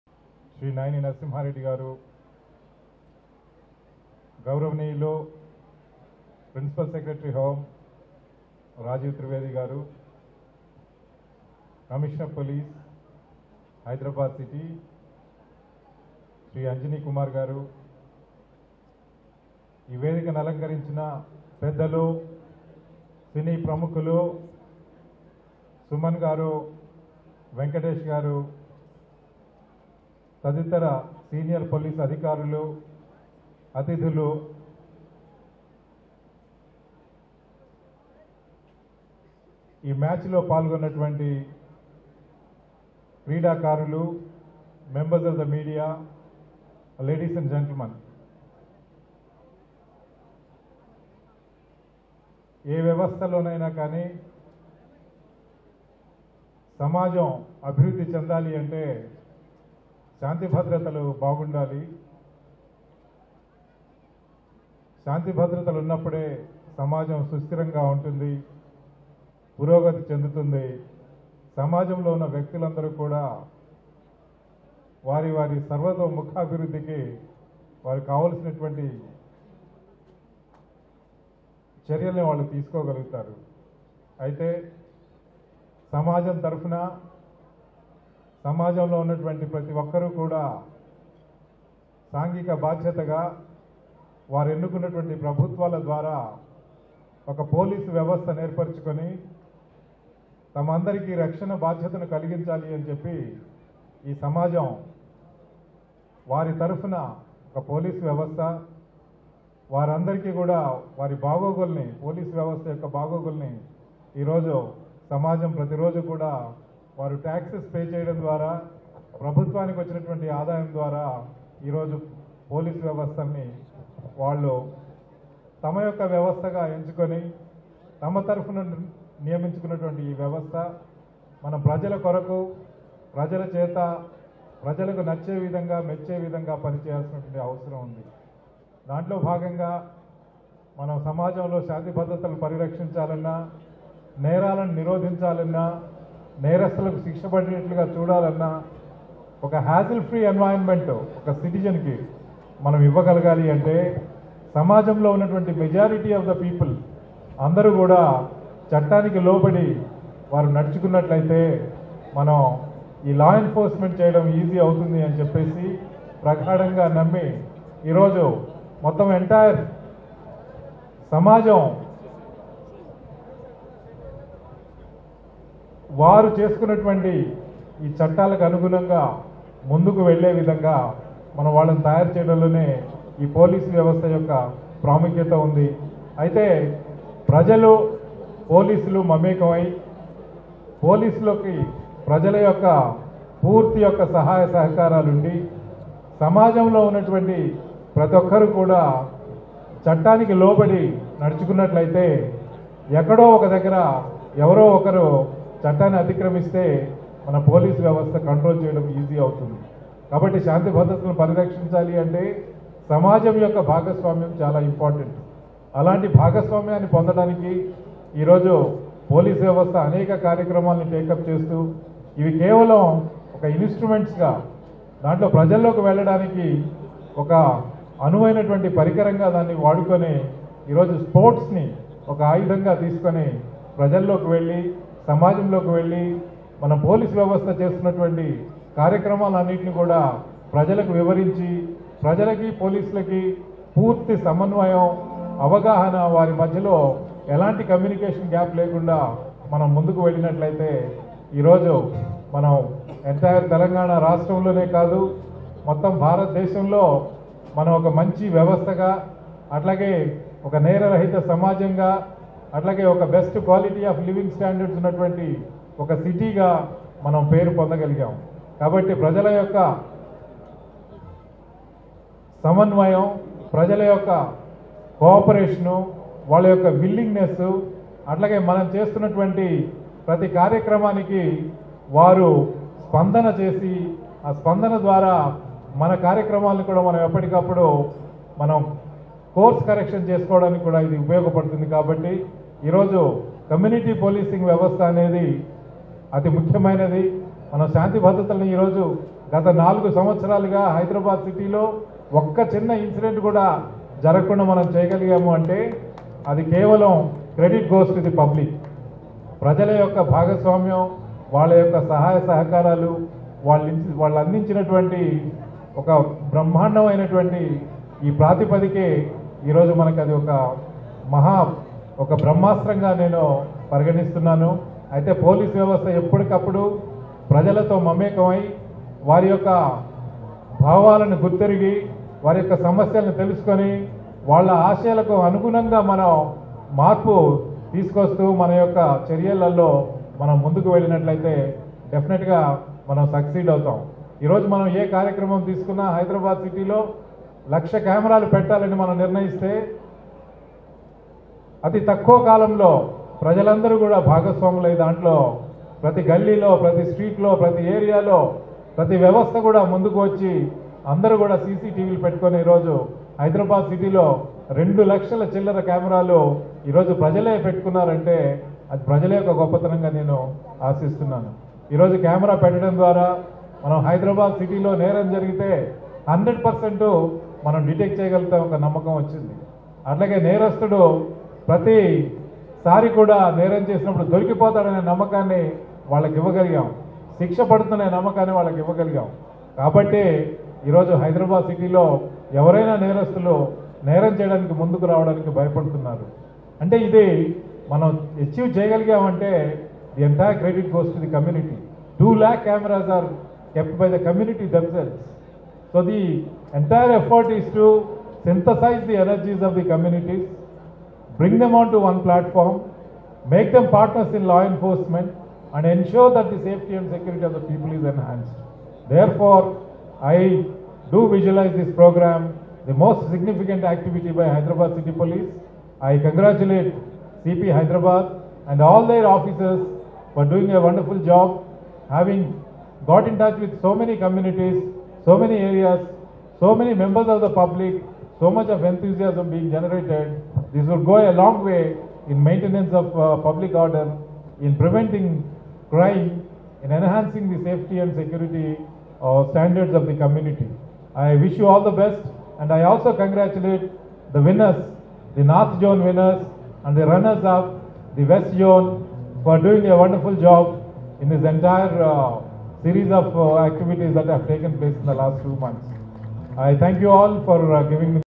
Hyderabad Police League-Sri.M.Mahendar Reddy,IPS,DGP TS speech/PRO HYDERABADPOLICE
Hyderabad Police League-2018 Grand Finale Cricket Tournament Match will be held tomorrow (3rd June, 2018) at 5pm in L.B.Stadium, Saifabad, Hyderabad. Sri.Nainini Narasimha Reddy,Hon’ble Minister for Home is Chief Guest,Sri.Rajiv Trivedi,IPS,Prl.Secretary to TS , Sri.M.Mahendar Reddy,IPS,DGP TS Guest of Honours and Special Invitees  and Cine Heros Sri.Akkineni Nagarjuna,Sri.Akhil,Sri.Daggubati Venkatesh,Sri.Nani, Sri.Suman celebrities also participated